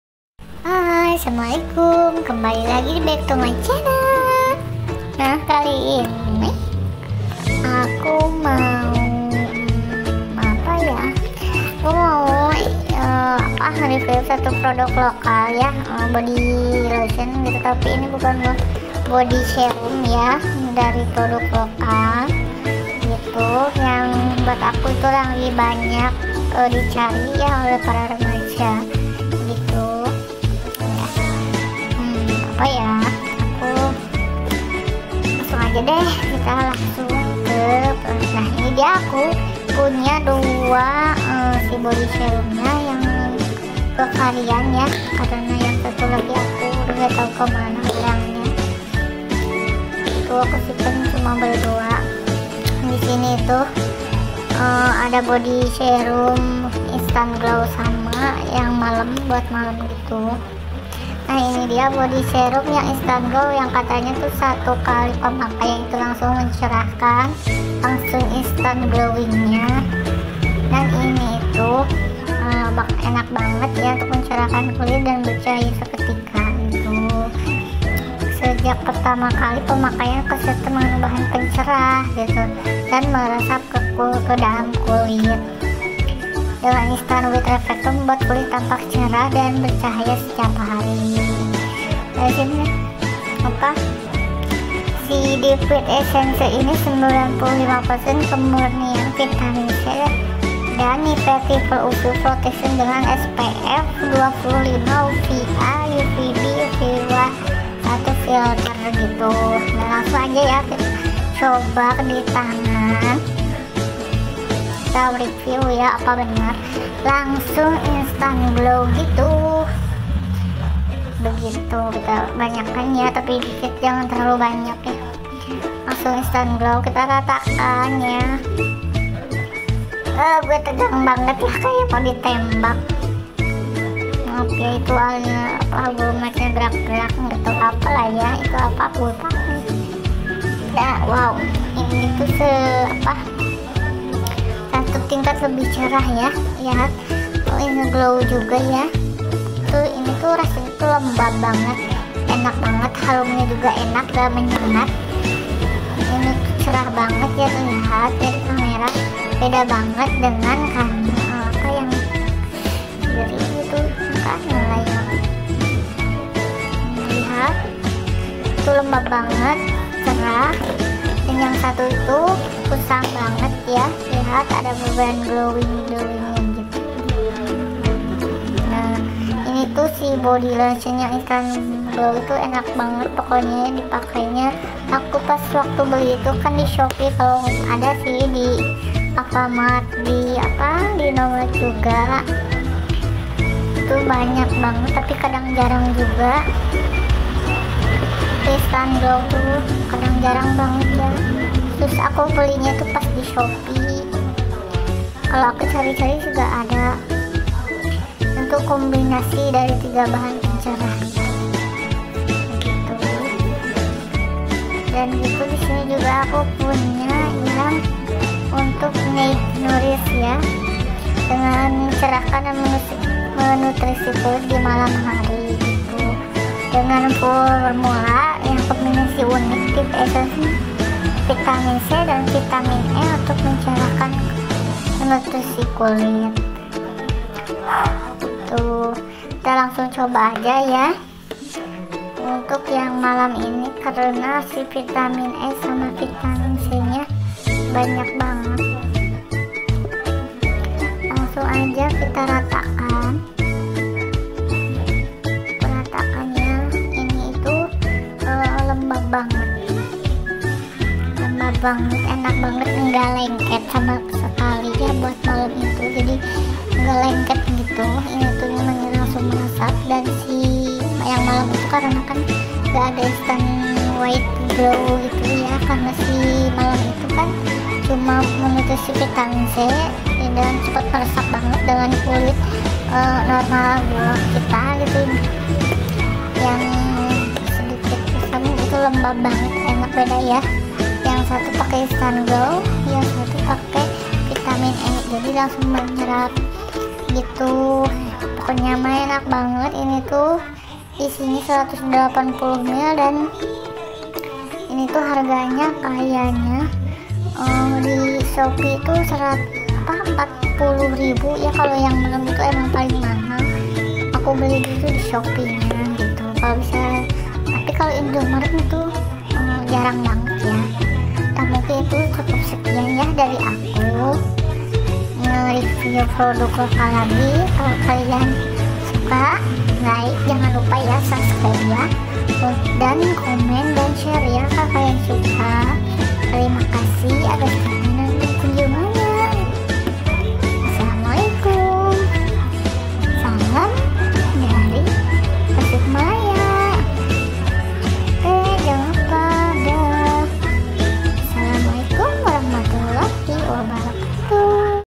REVIEW BODY SERUM NIVEA YANG LAGI BANYAK DI CARI REMAJA || Yuliaisnani channel
VIDEO ini menampilkan review produk produk lokal skincare remaja